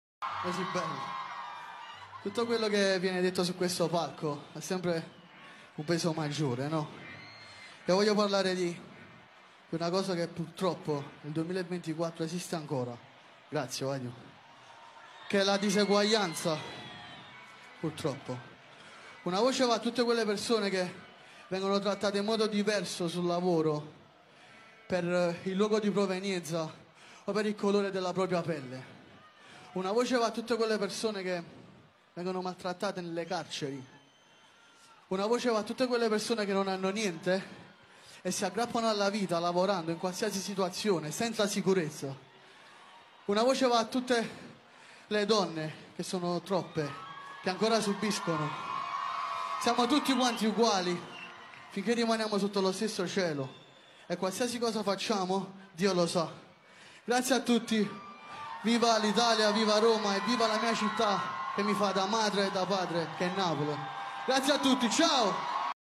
Il discorso di Geolier dal palco del concertone a Roma sulle discriminazioni
Tutto quello che viene detto su questo palco ha sempre un peso maggiore
Io voglio parlare di una cosa che purtroppo nel 2024 esiste ancora che è la diseguaglianza, purtroppo
Una voce va a tutte quelle persone che vengono trattate in modo diverso sul lavoro per il luogo di provenienza o per il colore della propria pelle
Una voce va a tutte le persone che vengono maltrattate nelle carceri
Una voce va a tutte quelle persone che non hanno niente e si aggrappano alla vita lavorando, in qualsiasi situazione, senza sicurezza
Una voce va a tutte le donne, che ancora sono troppe che subiscono
Siamo tutti quanti uguali finché rimaniamo sotto lo stesso cielo e qualsiasi cosa facciamo, Dio lo sa.
Viva l'Italia, viva Roma, evviva la mia città che mi fa da madre e da padre che è Napule